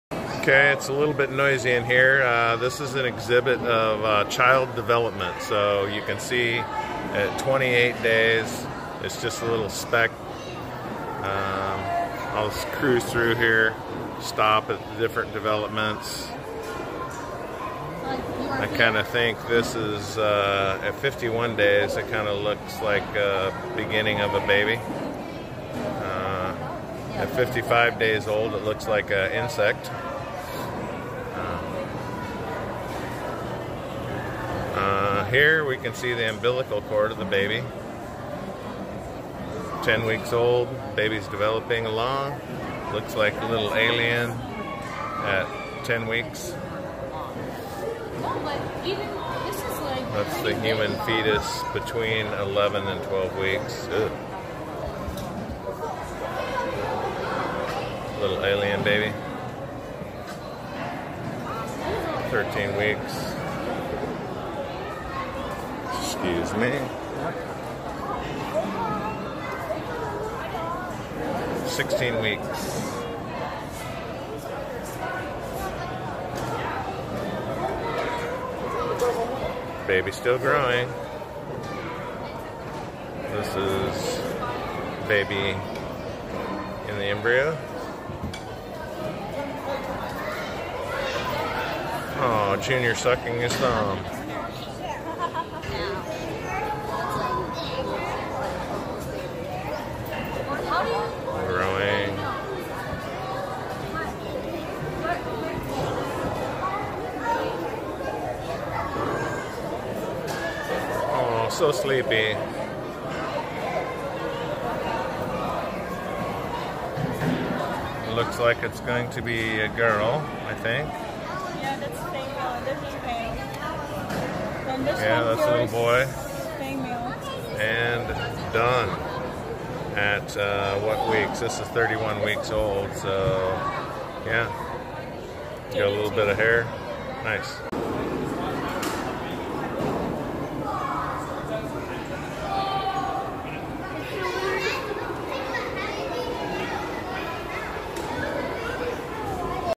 Prenatal Development
Each baby has a sign under it giving the age from fertilization of the child and what is going on for the baby developing.